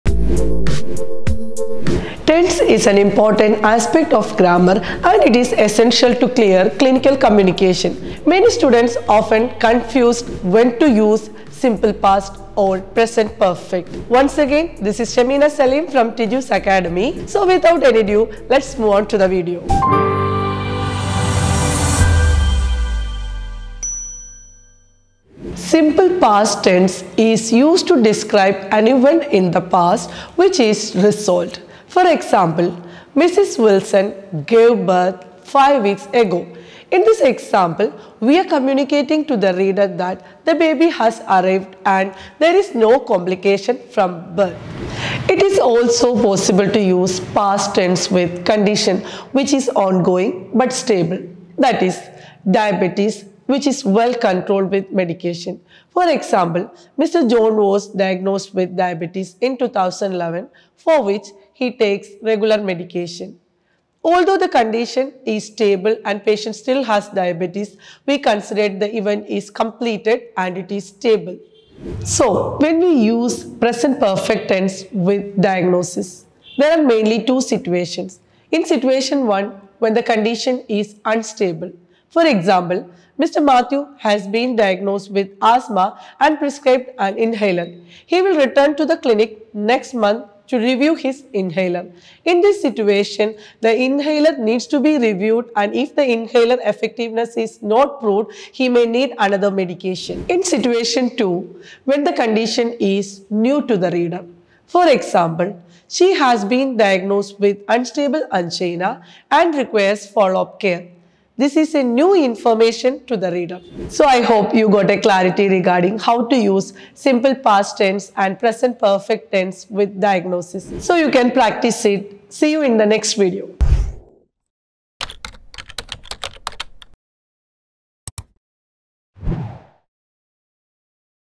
Past Tense Vs Present Perfect Tense | OET Writing Tips
In this video, we'll be discussing the difference between the Past Tense and the Present Perfect Tense in OET writing. We'll also give some tips on how to use these tenses correctly in your writing.
If you're new to the OET, or just want to improve your writing skills, then this video is for you! By the end of this video, you'll have a better understanding of how to use the Past Tense and the Present Perfect Tense in OET Writing.